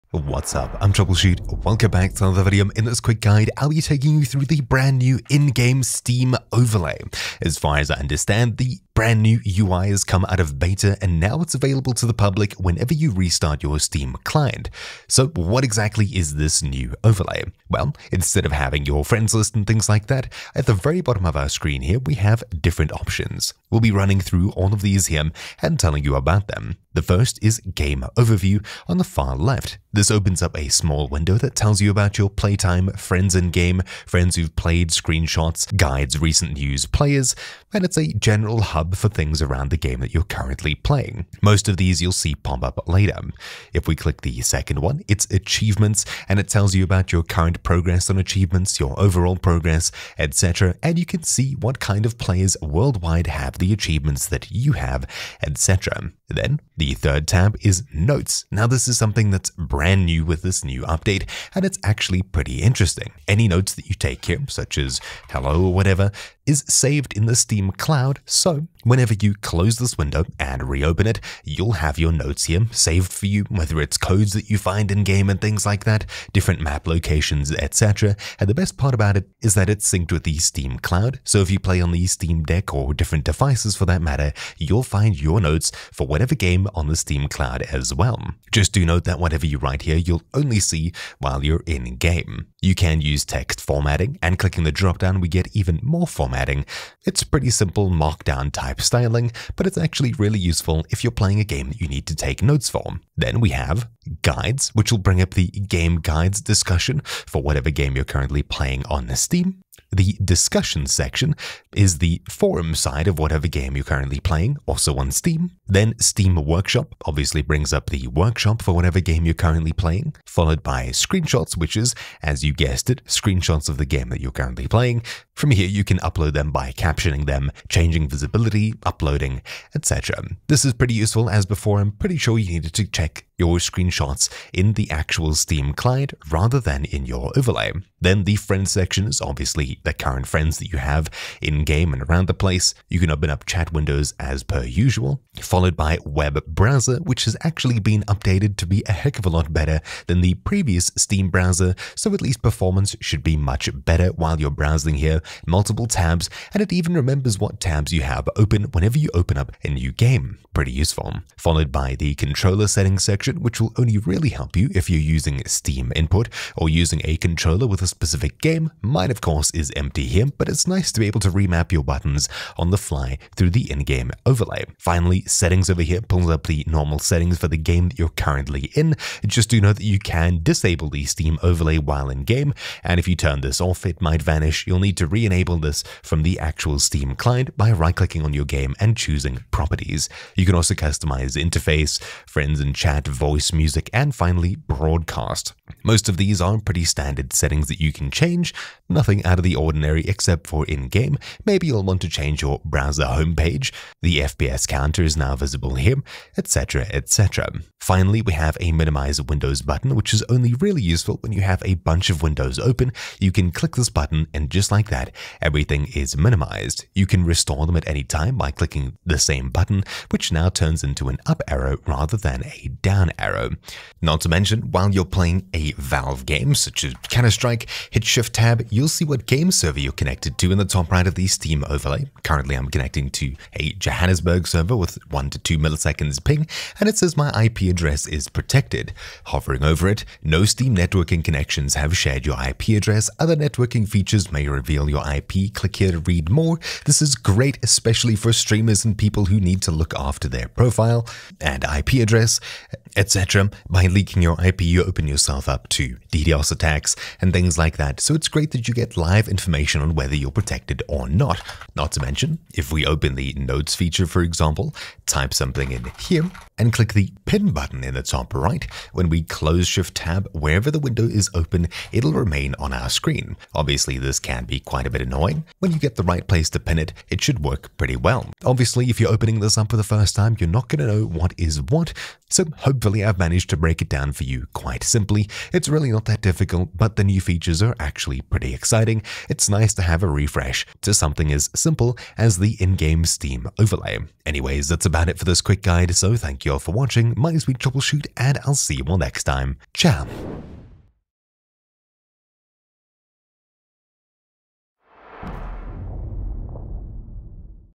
NEW Steam Overlay Breakdown & Guide | Steam UI Update
Steam's new overlay has rolled out to everyone with the coming update. The biggest noticeable overhaul is the in-game Steam Overlay. Confused about the new layout? This is a full breakdown of the new UI elements in the new Steam Overlay.

Timestamps:
0:00 - Explanation
0:15 - What changed
0:27 - Game Overview
0:43 - Achievements
0:56 - Notes (Steam Cloud)
1:39 - Guides
1:49 - Discussions & Steam Workshop
1:59 - Screenshots
2:18 - Friends
2:25 - Overhauled Web Browser
2:41 - Controller settings
2:53 - Settings
3:27 - Minimize windows